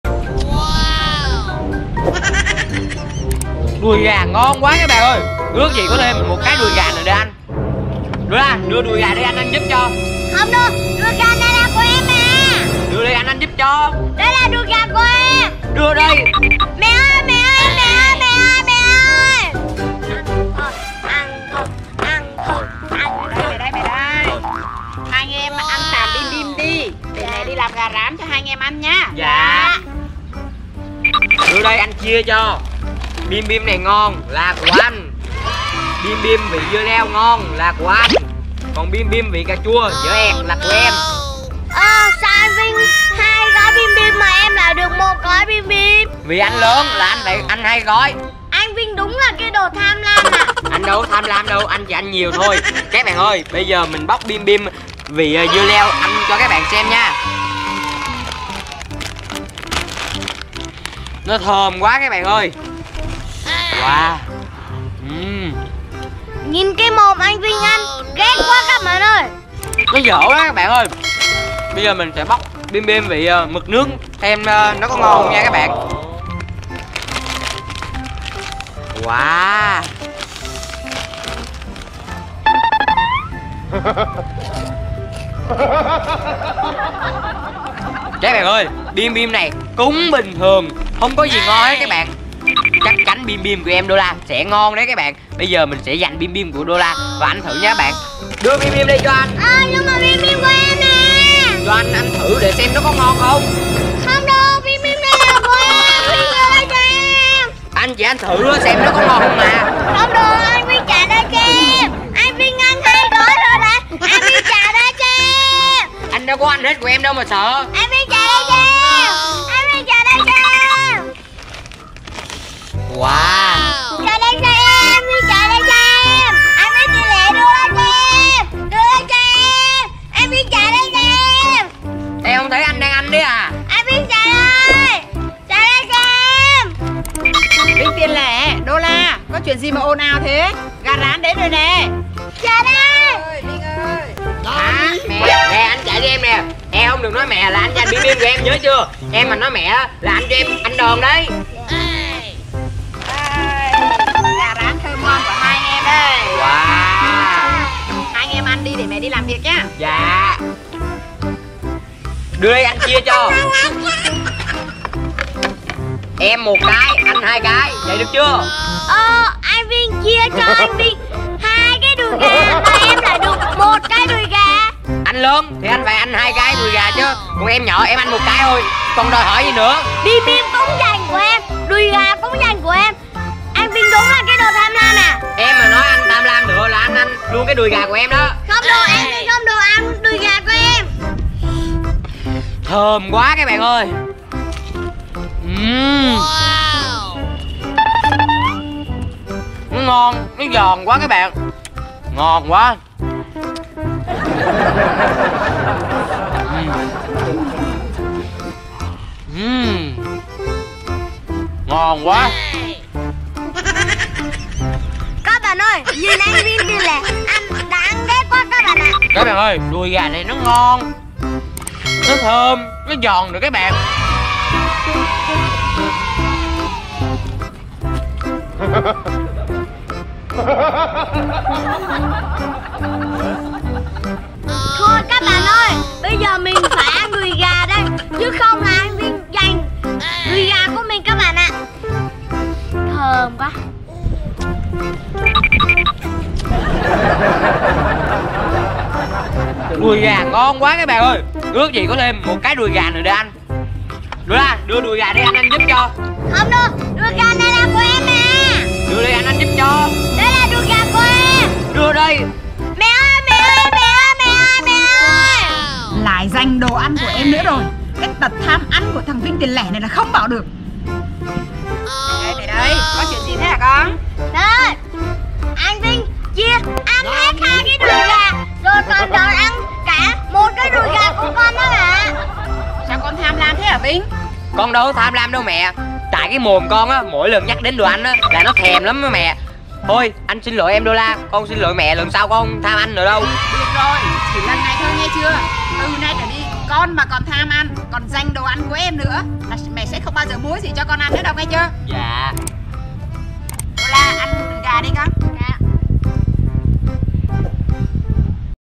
Gà Rán Của Em - Anh Trai Lanh Chanh Không Biết Nhường Nhịn Còn Tranh Giành Với Em! Thầy Thái Vlog
Thầy Thái Vlog Cảm ơn các bạn đã ủng hộ kênh!
# ThầyTháiVlog